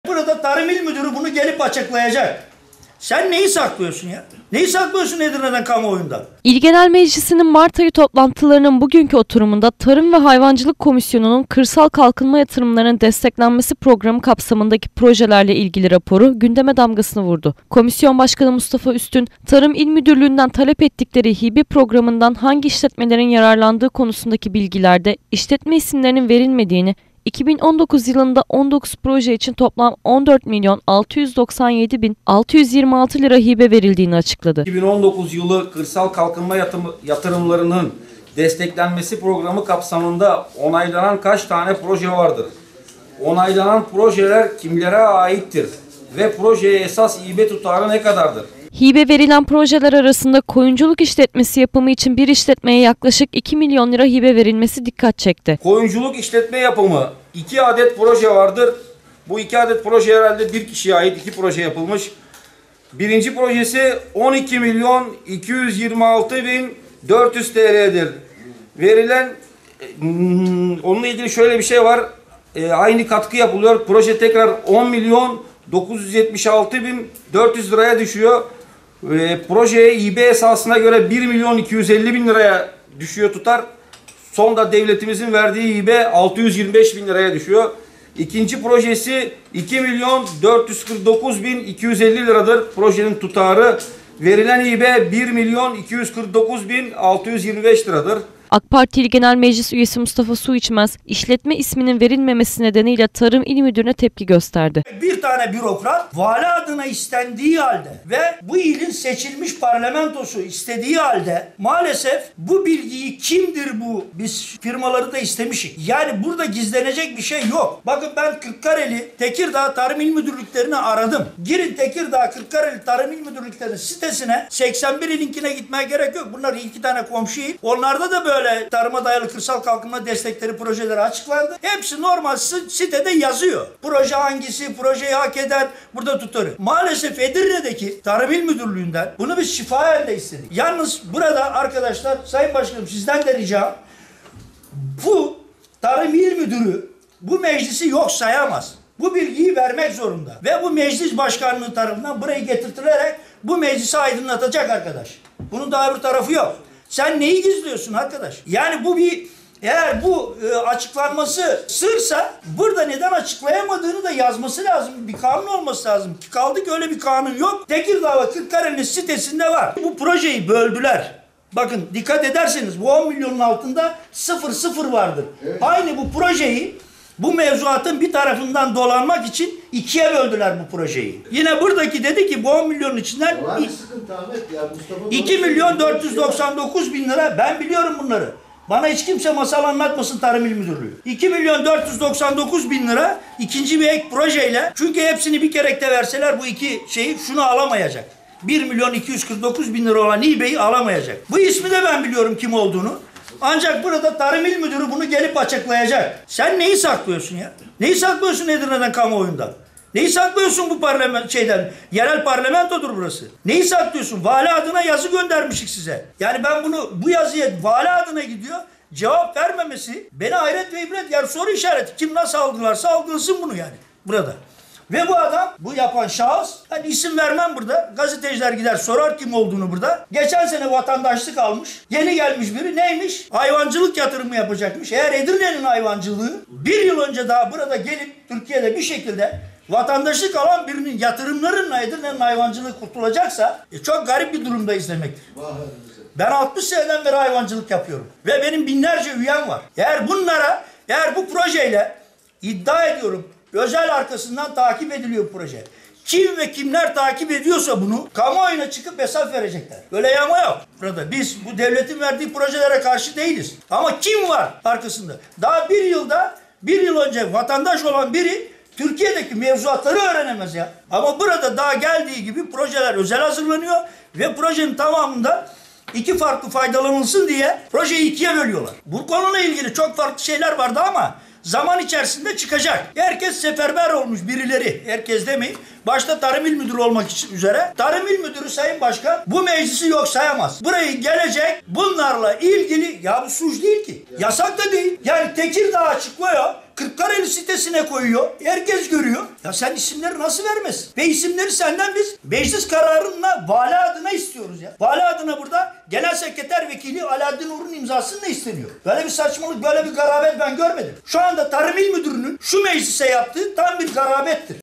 EDİRNE'DE BİR İŞLETMEYE 2 MİLYON LİRA HİBE
İl Genel Meclisi’nin Tarım ve Hayvancılık Komisyonu, Edirne’de Kırsal Kalkınma Yatırımları’ndan hangi projelerin yer aldığını açıkladı. Tarım İl Müdürlüğü’nün, Komisyon üyelerine projeden yararlanan işletmelerin isimlerini vermemesi, meclis üyelerinin tepkisine neden oldu. Komisyon raporuna göre Edirne’de yaklaşık 2 milyon lira hibe desteği alan işletmenin kime ait olduğu merak konusu oldu.